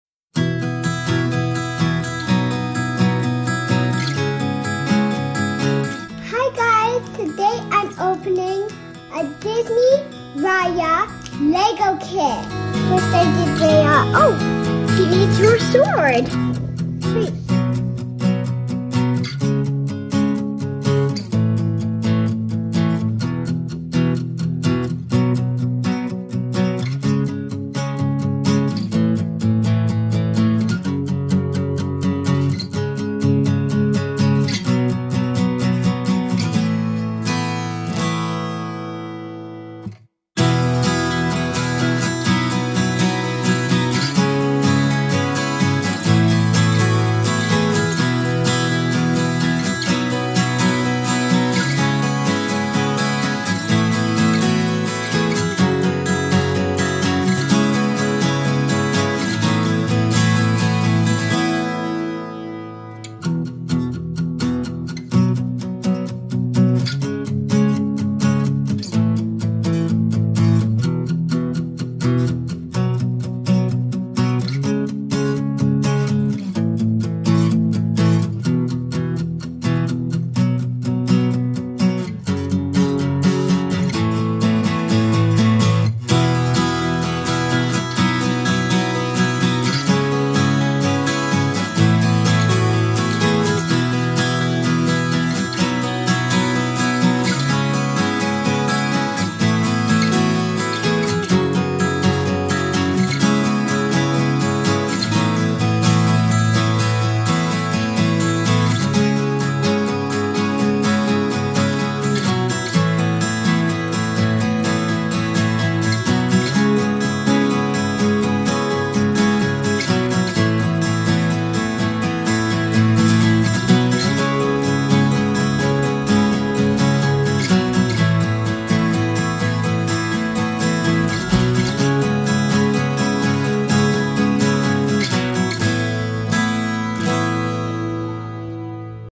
Disney Raya LEGO Polybag 30558 | Quick how to video for kids
Check out this quick and easy video on how to assemble a Disney Raya and the Last Dragon LEGO polybag (LEGO number 30558) collectible playset.